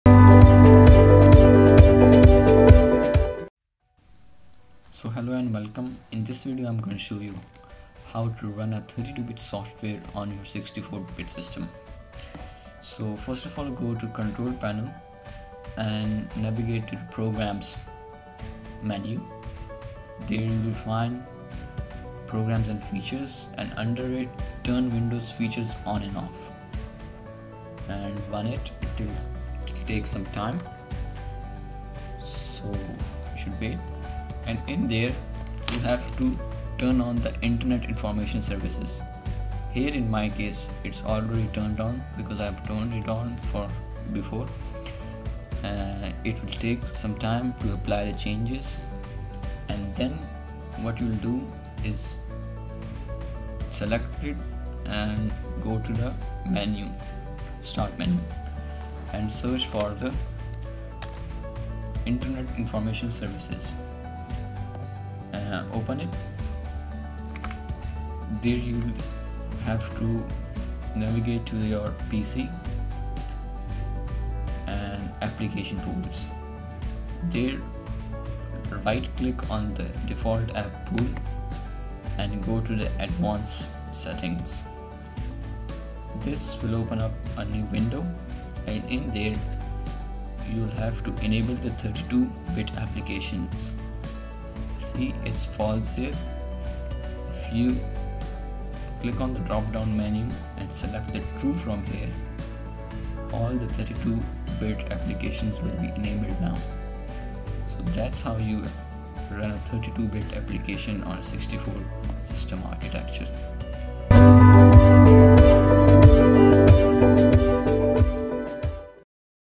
Running 32 bit Program/software on a 64 bit Windows System
The Versatile Engineer presentation
In this video, you will learn how to run a 32-bit architectured windows program or software on a 64-bit system, bear in mind that a 64-bit system can run 32-bit software but not vice versa.